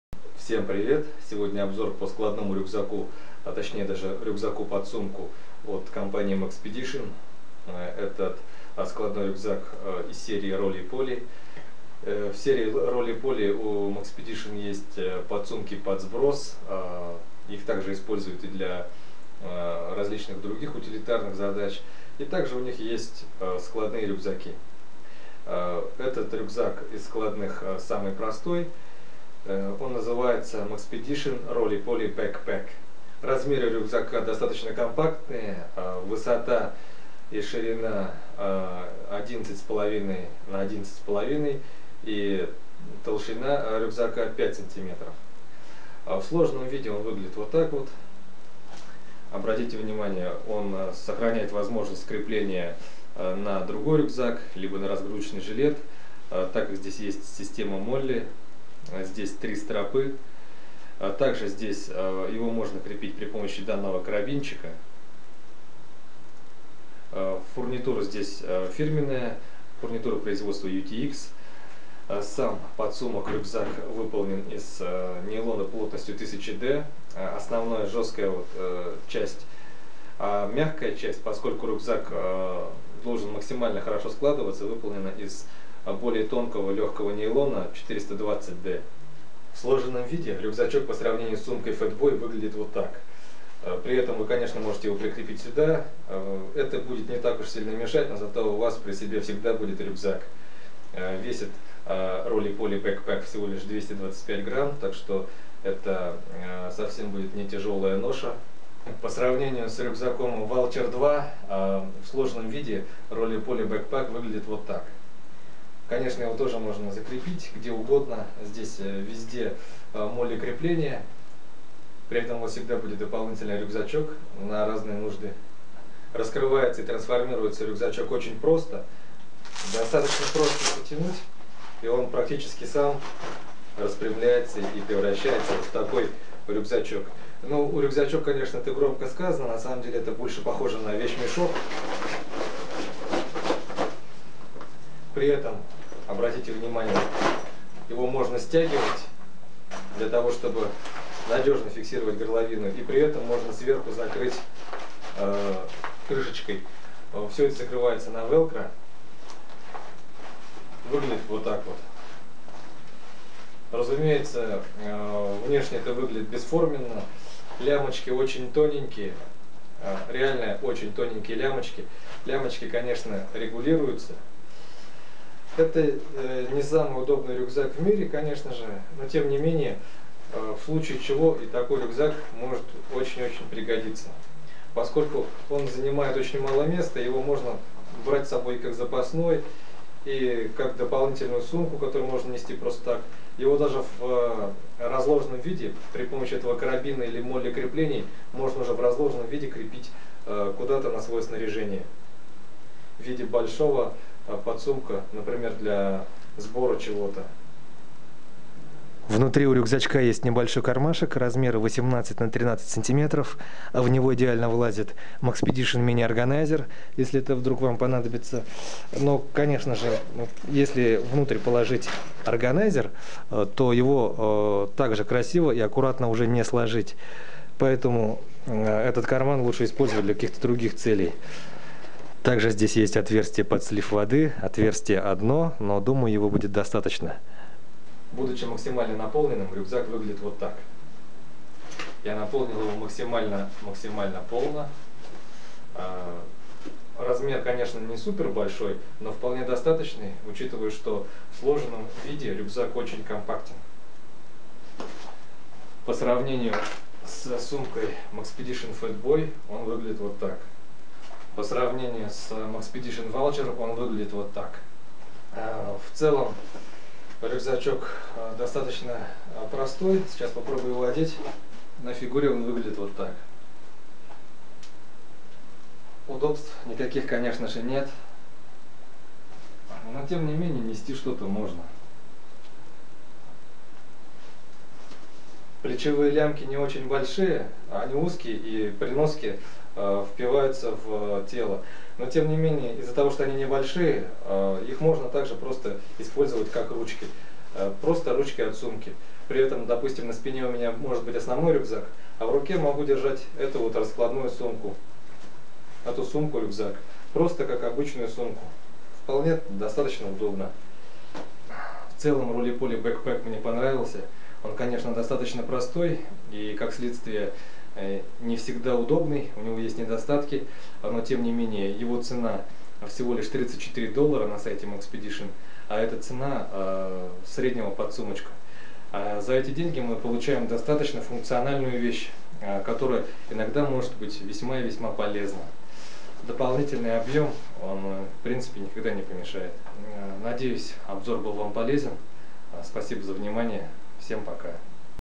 Обзор Maxpedition ROLLYPOLY Folding Backpack
Вполне функциональный складной рюкзак. Есть недостатки, но это все в угоду размерам и цене.